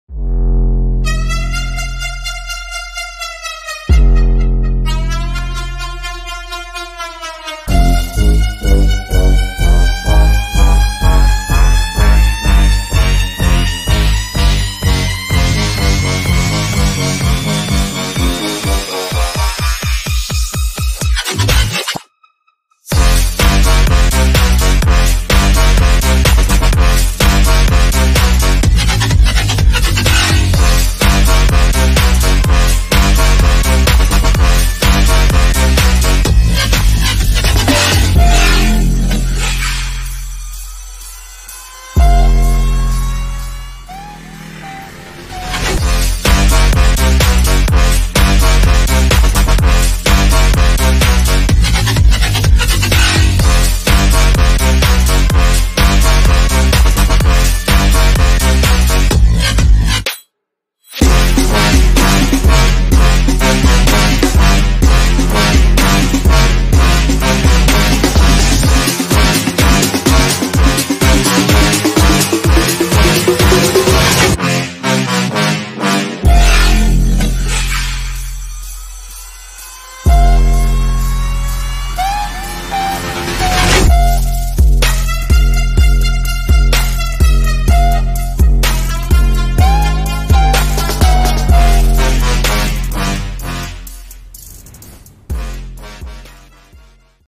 Kiddos first long steep ride! Start them young!
As an overseas worker and a father. Bonding with kids is very important to me. Cycling is the my way to have close bonding with my kids.